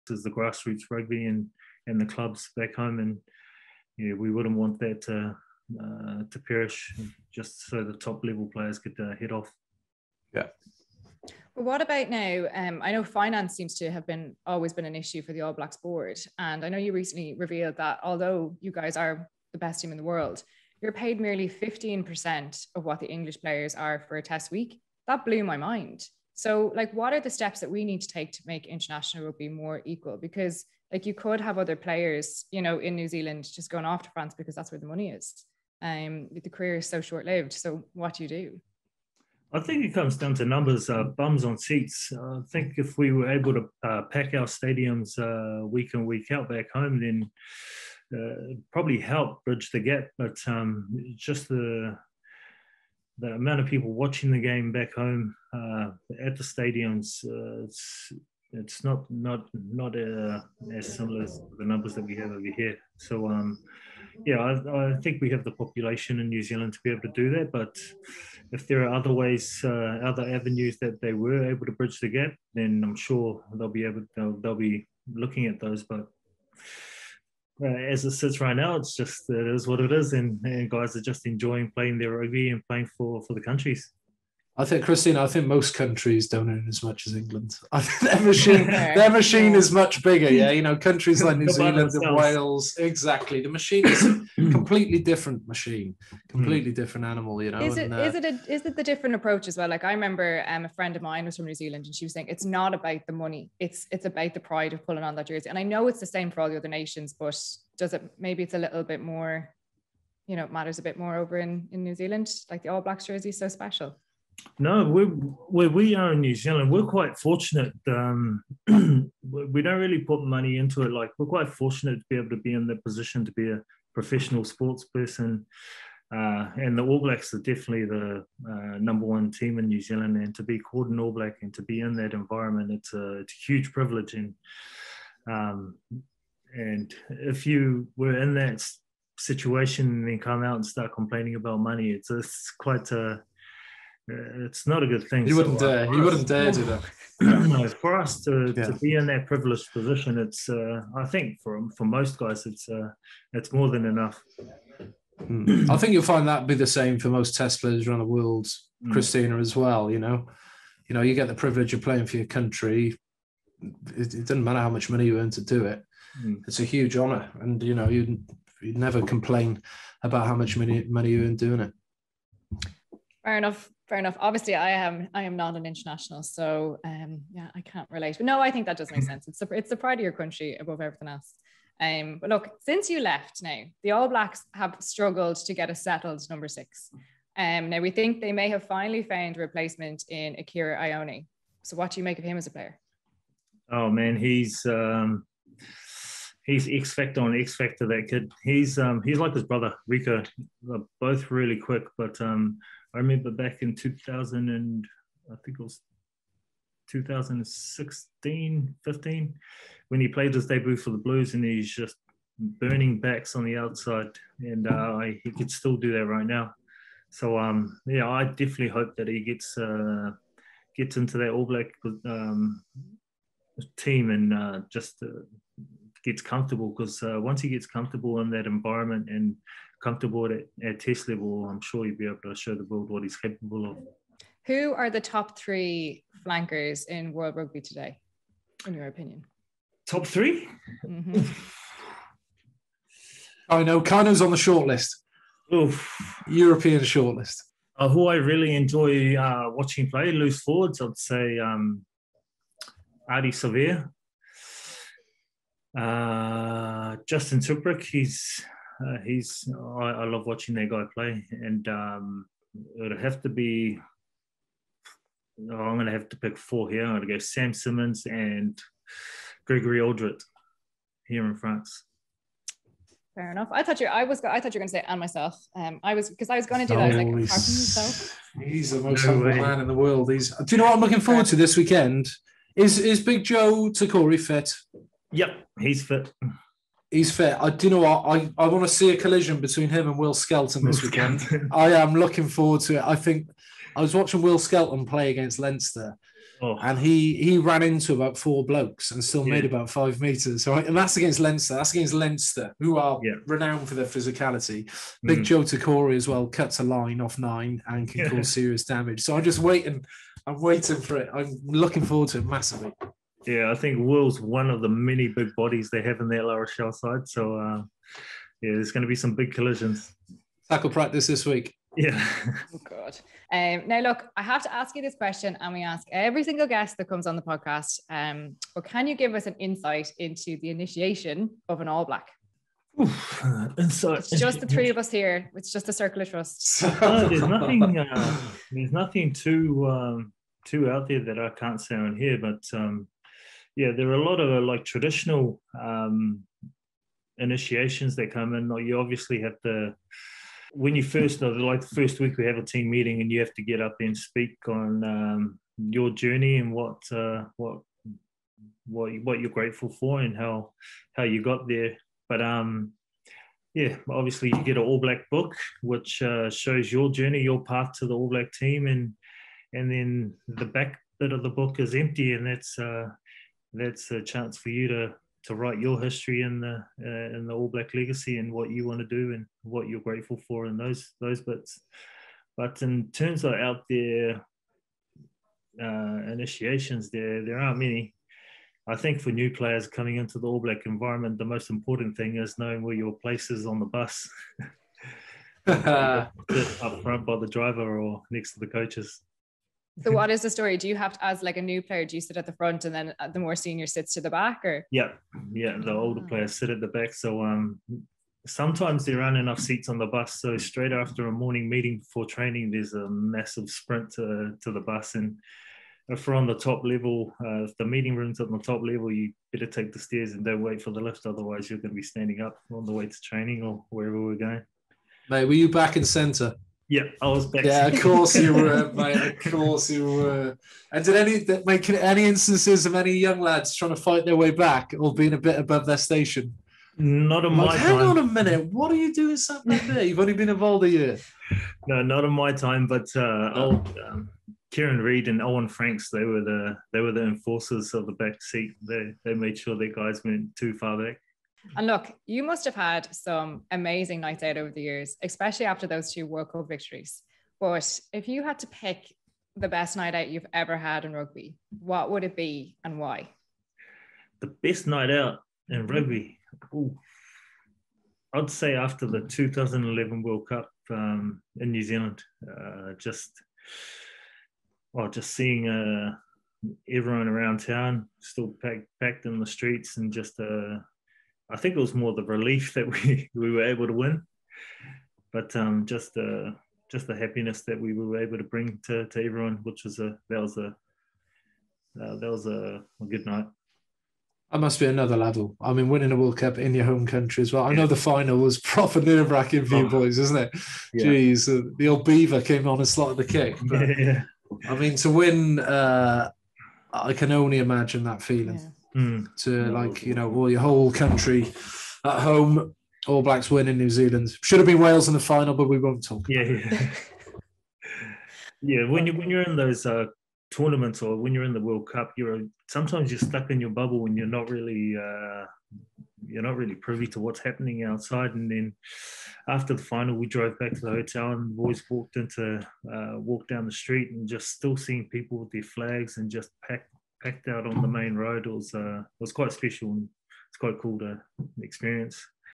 Who are the top flankers in rugby right now? - Jerome Kaino | RugbyPass Offload
New Zealand rugby legend Jerome Kaino gives us his take on the top flankers in world rugby at the moment, excluding himself!